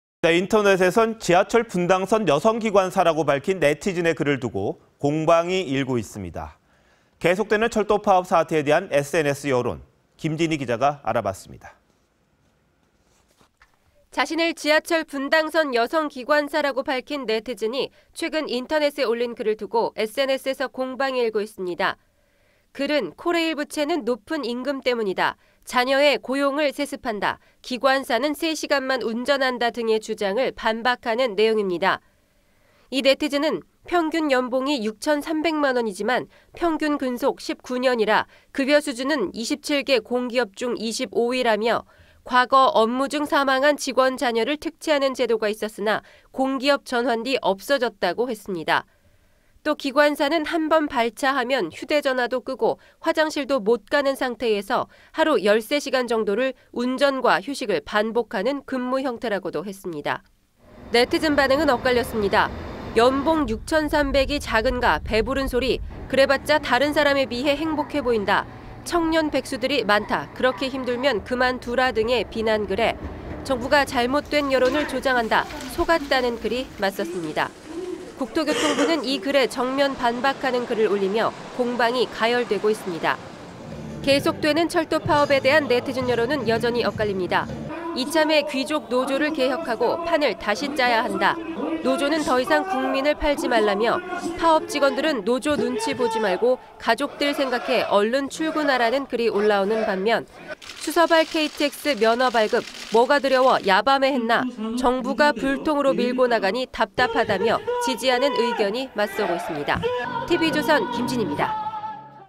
'분당선 여성 기관사 글' SNS 논란
인터넷엔 지하철 분당선 여성 기관사라 밝힌 네티즌의 글을 두고 공방이 일고 있습니다. 또 계속되는 철도 파업 사태에 대한 SNS 여론, 김진희 기자가 보도합니다.